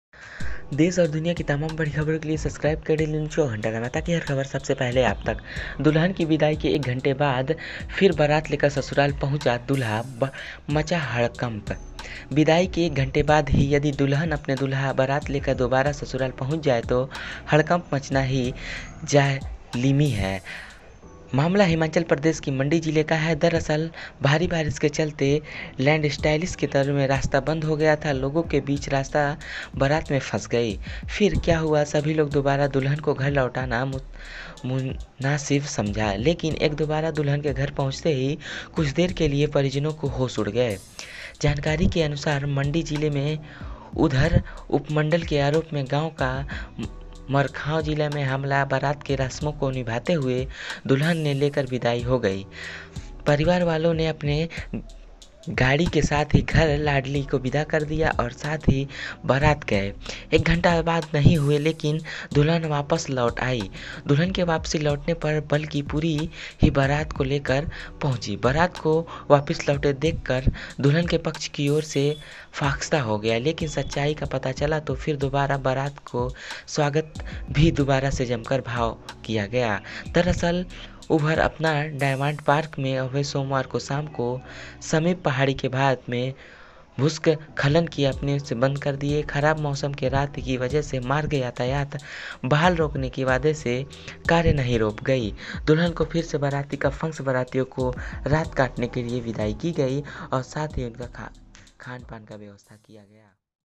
दुल्हन की विदाई के 1 घंटे बाद ही फिर बारात लेकर ससुराल पहुंचा दूल्हा, मचा हड़कंप
विदाई के एक घँटे बाद ही यदि दुल्हन और दूल्हा बारात लेकर दोबारा ससुराल पहुंच जाए तो हड़कंप मचना तो लाजिमी है. मामला हिमाचल प्रदेश के मंडी जिले का है. दरअसल, भारी बारिश के चलते लैंडस्लाइड हुई और रास्ता बंद हो गया. लोगों के साथ-साथ बारात भी फंस गई. फिर क्या था सभी ने दोबारा दुल्हन के घर लौटना ही मुनासिब समझा. लेकिन जब दोबारा दुल्हन के घर पहुंचे तो कुछ देर के लिए परिजनों के होश उड़ गए.जानकारी के अनुसार, मंडी जिले के पधर उपमंडल के रोपा गांव में मरखान गांव का यह मामला है. बारात सारी रस्मों को निभाने के बाद दुल्हन को लेकर विदा हो गई थी. परिवार वालों ने भी अपने घर की लाडली को विदा कर दिया, लेकिन अभी बारात को गए हुए एक घंटा भी नहीं हुआ था कि दुल्हन वापिस लौट आई. दुल्हन ही वापिस नहीं लौटी, बल्कि पूरी की पूरी बारात भी लेकर पहुंची. बारात को वापिस लौटता देख दुल्हन पक्ष के होश फाख्ता हो गए, लेकिन जब सच्चाई पता चली तो फिर दोबारा से बारात का स्वागत भी हुआ और जमकर आव भगत भी हुई.